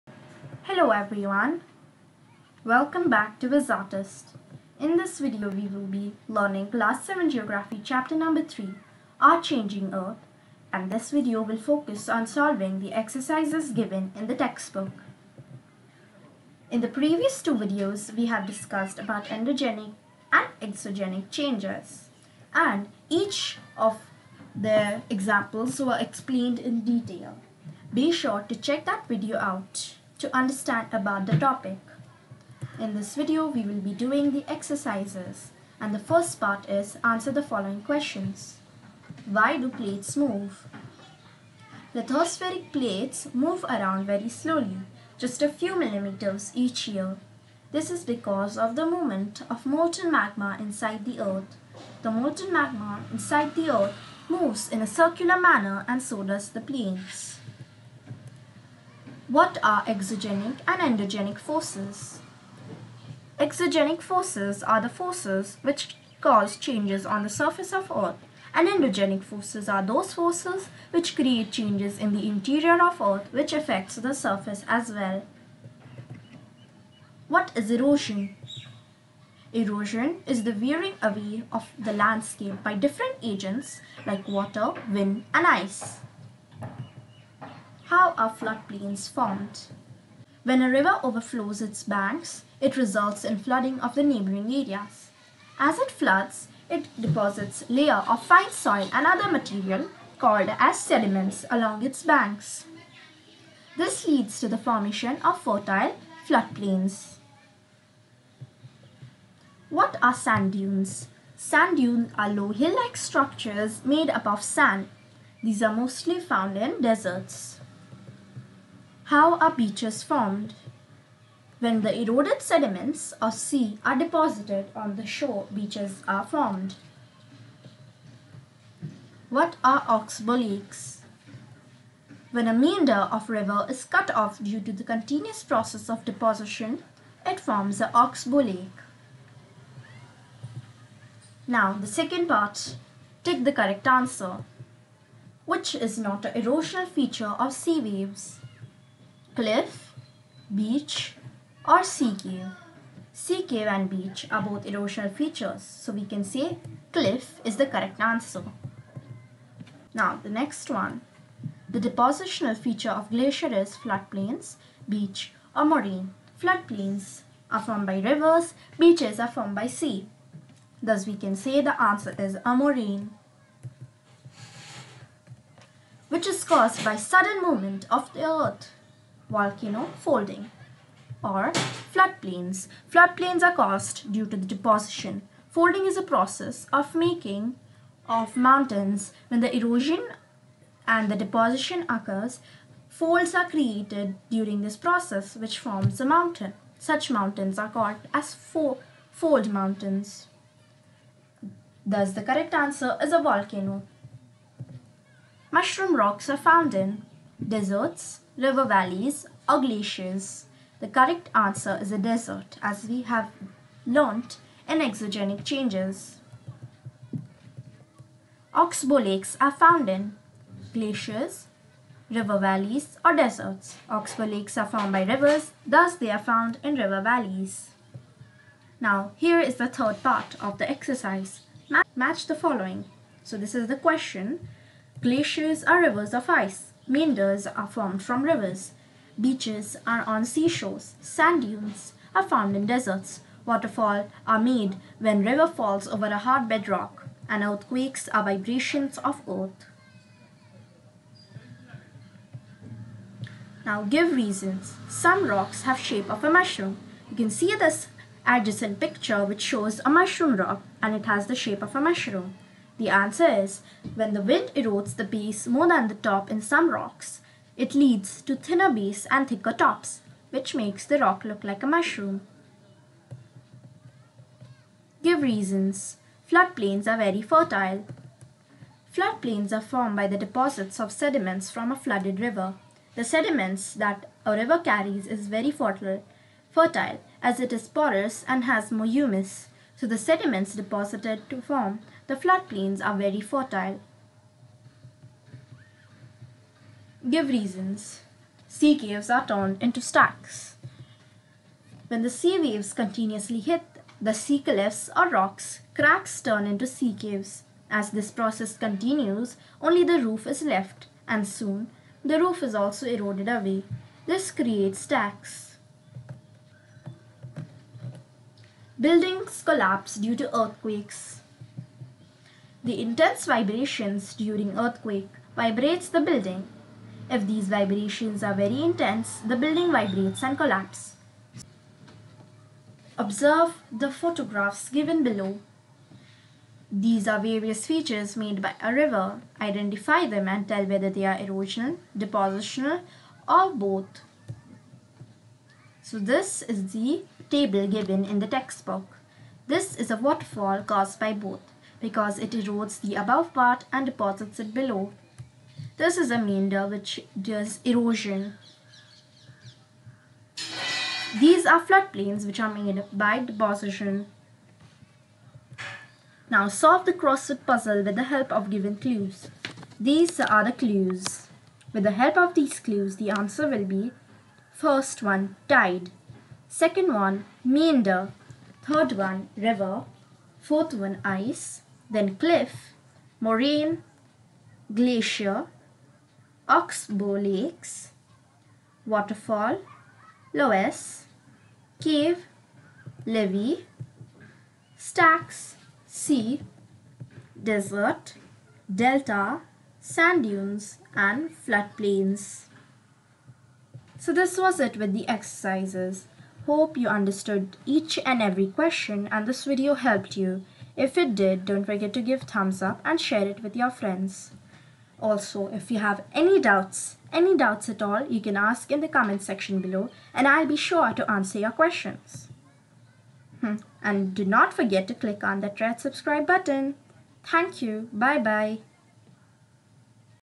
Our changing earth - T.B exercise || Exogenic Changes || Class 7 Geography || Wizartist
Hi all
So the previous 2 uploads explained chapter 3 of class 7 geography which gives a detailed understanding of the changes on earth. This video will be discussing the exercises of the chapter give in the TB.